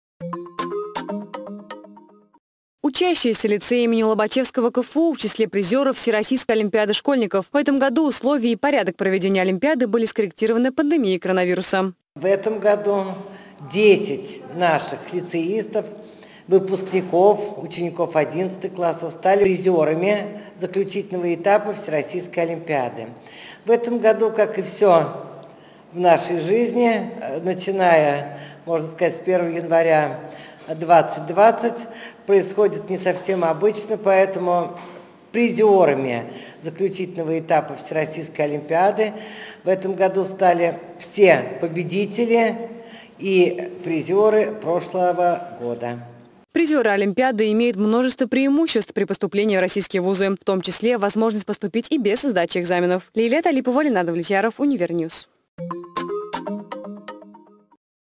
ВСЕРОССИЙСКАЯ ОЛИМПИАДА ШКОЛЬНИКОВ
Тем временем учащиеся Лицея имени Лобачевского КФУ стали призерами Всероссийской олимпиады школьников 2020 года. Что, впрочем, неудивительно — из года в год лицеисты завоевывают первые места на олимпиадах различного уровня сложности.